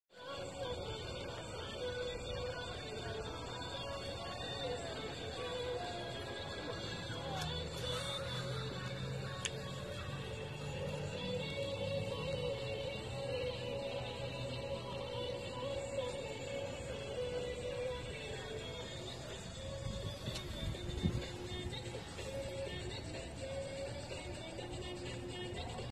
Ice carving at Fire and Ice Festival. Radio inside
Fire and Ice festival in Frankfort with Shine 99